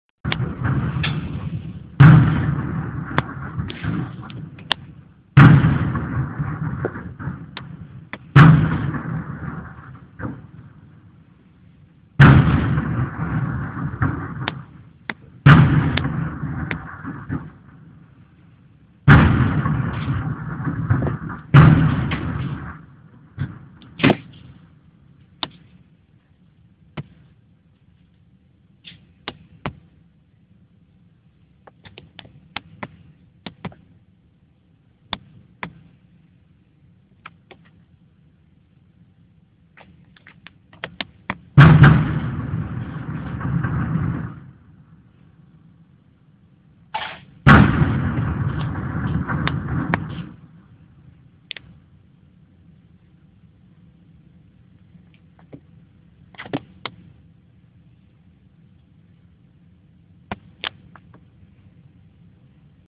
A surprise for one of the Thames River cruise boats!
Video0024.3gp - Second gun salute from HMS Belfast in London (in the evening on the day the Queen opened Parliament).